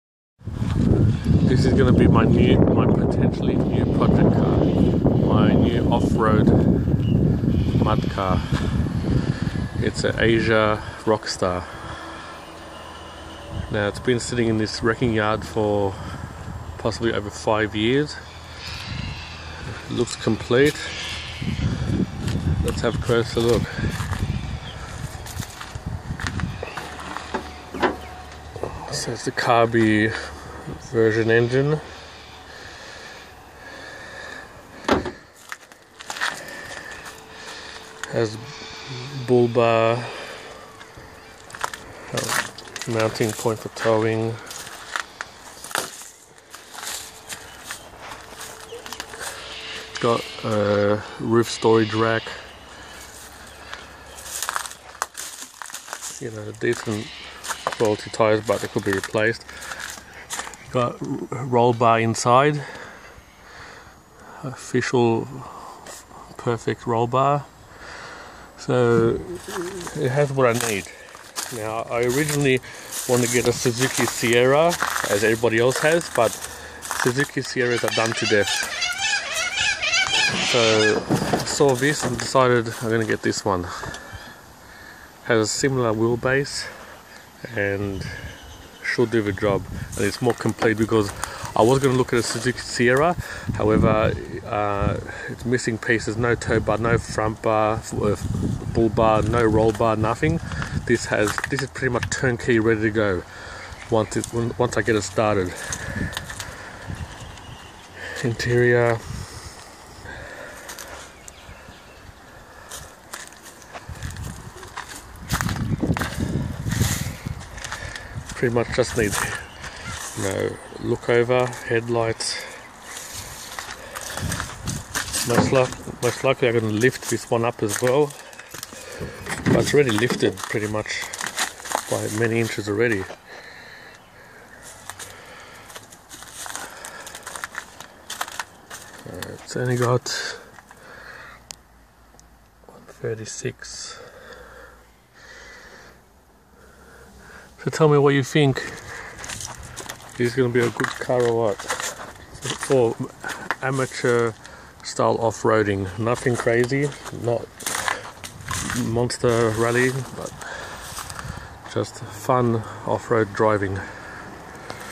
Rocsta offroad project Part 1
I hereby show you my new offroad 4wd project car, The Rocsta.

I bought the rocsta from a wrecker for a reasonable low price. It was sitting for many years. I actually walked past this car many times in the past, and at some moments, i would look inside it and see how complete it looked. Who knew i would end up buying it etc.

Anyway, i was able to spin the engine manually, the timing belt is still in place, and it holds coolant, thats reassurance so far at this point in time. In upcoming videos, i will show it starting/running and do off road driving and exploring in the bush etc. It will be a journey.

Plans:
Lift the car by approx 2 to 4 inches.
Fit muddies
Snorkel intake

Thats pretty much it.

Story as follows:
I always had a thing with offroading. I always wanted to try it out, however, never had the time to do so. I am now entering the world of 4wd offroading exploring.

And just recently, i saw 3 blokes rock climbing a very steep slope in the bush near me. Was so entertaining. The 4wd's offroading where a big beast Landcruiser, jeep cherokee and a suzuki jimny. After watching them drive up the mountain, I WAS HOOKED.

Having said this, i always like the suzuki sierra, cos its small and nimble, however, its been done to death. Everyone knows and has a suzuki sierra. I almost bought one, but since i saw this one, i had to get it. You dont see to many Roksta's around.